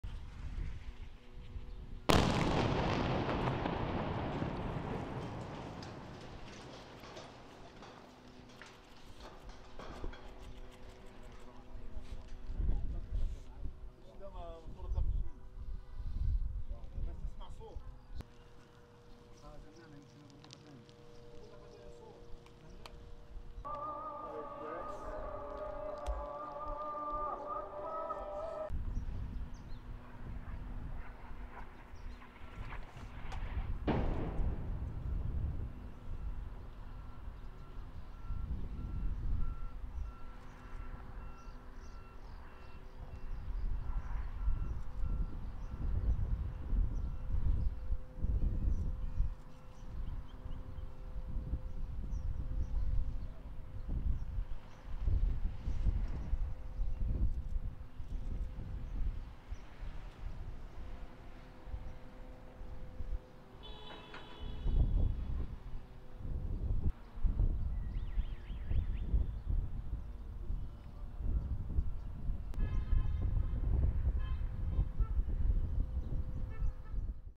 Latest airstrike by Israeli forces on Gaza
Israeli air raids continued on Saturday in Gaza, after one raid killed at least 10 Palestinians in the deadliest single strike since the battle with Gaza's militant Hamas rulers erupted earlier this week. (May 15)